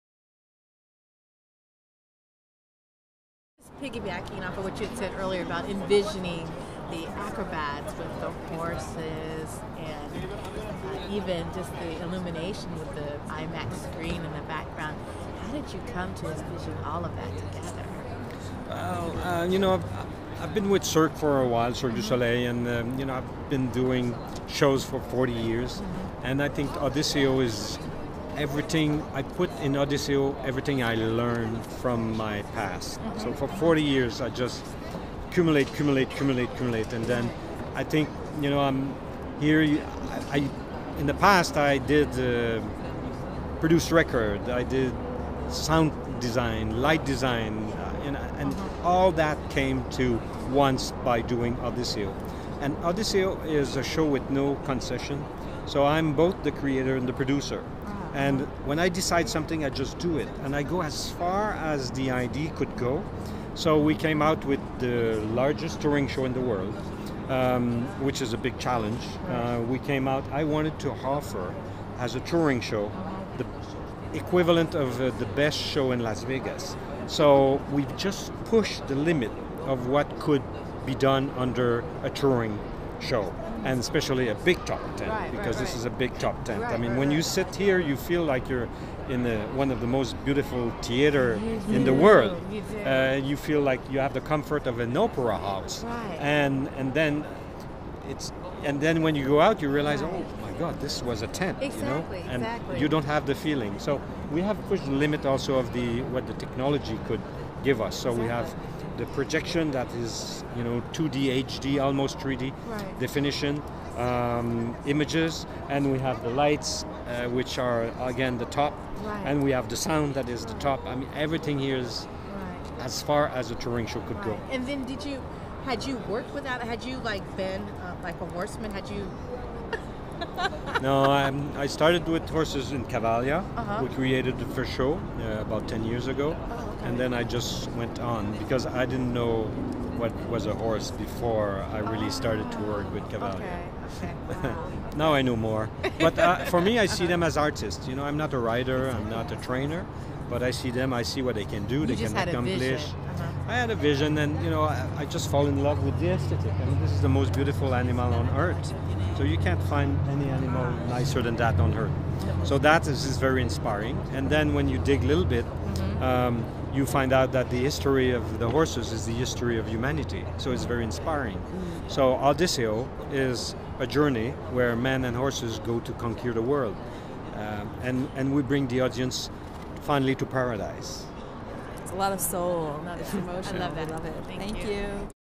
Cavalia Odysseo Normand Latourelle
We talked with Cavalia creator Normand Latourelle at the Odysseo media preview in Burbank, CA